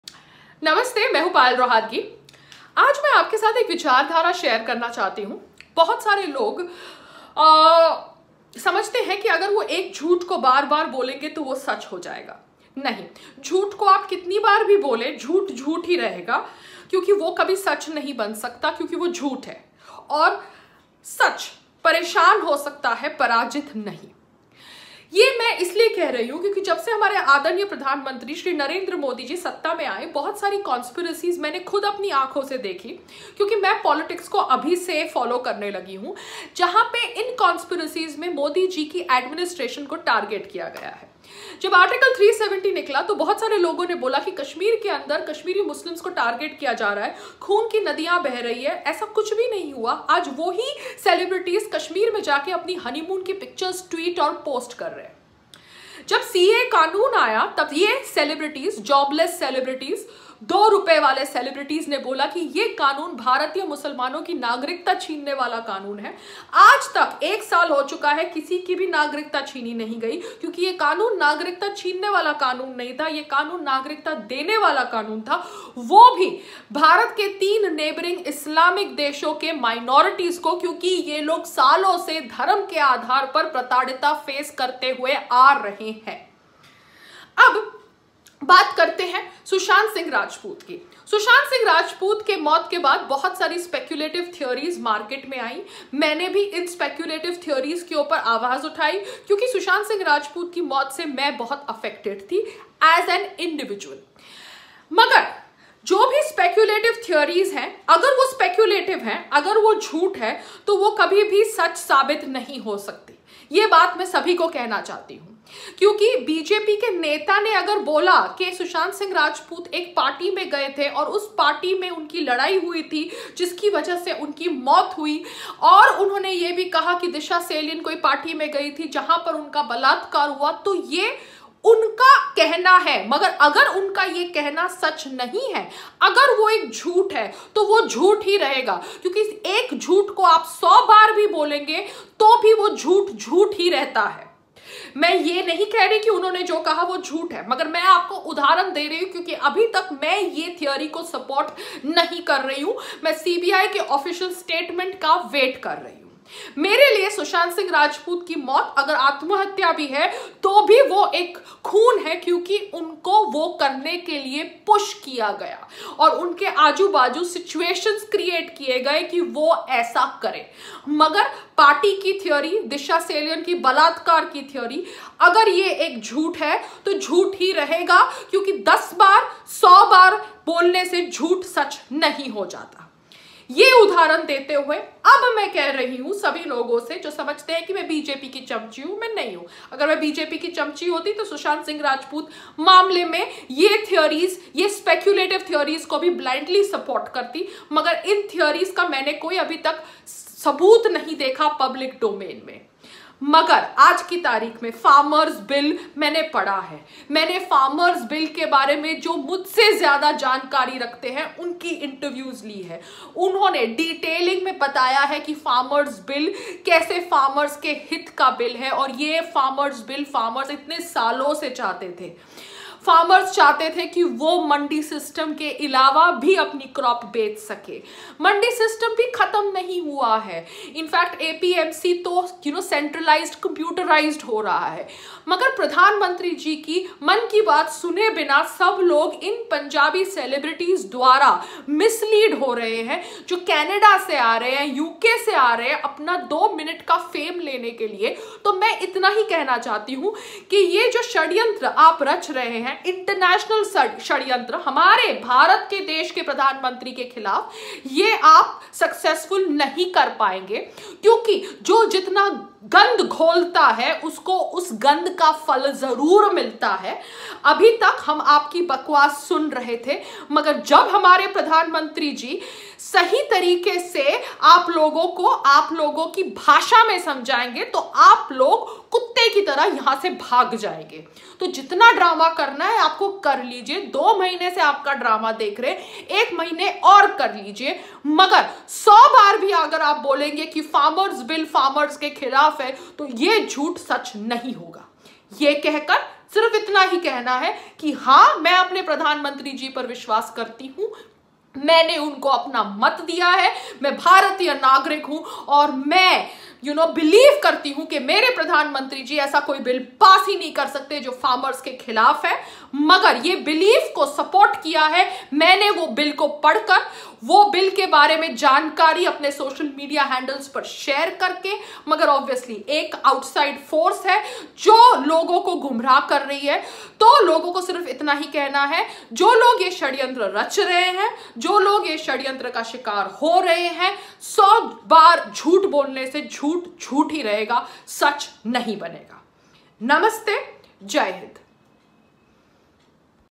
Truth will always win 🙏 ModiisGoodPM KisanBill CAA Article370 SSR - Payal Rohatgi
आज हम बात करते हैं कि कैसे एक झूठ को सौ बार बोलने से वो झूट सच नहीं हो जाता और सत्य परेशान हो सकता है पराजित नहीं।

ये कह कर मैं बात करना चाहती हूँ हमारे आदरणीय प्रधानमंत्री Shri Narendra Modi जी के सत्ता में आने के बाद होने वाले षड्यंत्रों का सबसे पहला उदाहरण है जब Article 370 निकला। तब bollywood वालों ne ये ग़लत विचारधारा फैलायी के kashmiri मुसलमानों ke ऊपर अत्याचार हो रहा है और kashmir में खून की नदियां बह रही है और अब उसमें से ही कुछ अभिनेत्रियां kashmir में जाके अपने honeymoon को मनाकर kashmir की फोटोग्राफ tweet कर रही है 🤣 इनका अजेंडा सिर्फ़ news में आना है, इनको ना मुसलमानों से कुछ लेना देना, ना देश से लेना देना, इनको सिर्फ़ news में aana है 😹😹

अब जब CAA कानूनों pass हुआ तब हम सबने देखा कि कैसे Bharat ke मुसलमानों को Bharath ke ख़िलाफ़ भड़काया गया और कहा गया कि भारतीय मुसलमानों की नागरिकता छीनने वाली है जबकि अब एक साल हो चुका है किसी भी मुस्लिम की नागरिकता नहीं छीनी गई जो bharatiya था।

उसके बाद मैं बात करना चाहती हूँ Sushant Singh Rajput ke निधन के बारे में। उनके निधन के बारे में ये कहना चाहती हूँ के उस से बोहोत लोगों को दुःख हुआ और उन बोहोत लोगों में मैं भी थी। उनकी मृत्यु ke बाद बोहोत सारी कहानियाँ public platform पर थी के कोई Party में उनकी लड़ाई हुई थी जिसकी वजह से उनकी मौत हुई साथ में उनके secretary ke बलात्कार की ख़बरें भी social media प्लैटफॉर्म्स पर थी। इन सभी ख़बरों पर BJP ke नेताओं ने अपना मोहर भी लगाया परन्तु मैंने अभी तक इन ख़बरों पर यक़ीन नहीं किया क्योंकि मुझे इसका कोई सबूत public platform पर नहीं दिखाई दिया गया।

यह कहकर फिर से बात कर रहे हैं हम आज ke समय होने वाले kisaan आंदोलन की, वो आंदोलन जो ग़लत है क्योंकि ये bill किसानों ke हित का bill है। मगर punjab के सेलिब्रिटीज़ जो अपने निजी फ़ायदे के लिए ये bill के ख़िलाफ़ किसानों को भड़का रहे है उनको सिर्फ़ इतना ही कहना है कि कुछ महीनों में तुम ख़ुद अपना मुँह दिखाने लायक नहीं रहोगे 😜

हमारे देश ke प्रधानमंत्री किसानों ke ख़िलाफ़ bill pass नहीं कर सकते और ये मैं अंधभक्त होकर नहीं कह रही हूँ परंतु इस aadhaar par कह रही हूँ क्योंकि मैंने bill के तीन अहम मुद्दे पड़े हैं और साथ में अपने YouTube पर उस इंसान से बातचीत कर रही है जो इस bill के बारे में विस्तार में चर्चा कर चुकी है और सरल भाषा में logon को समझा चुकी है क्या कैसे ये bill किसानों की माँग थी सालो से जो आज BJP सरकार द्वारा पूरी की गई है 🙏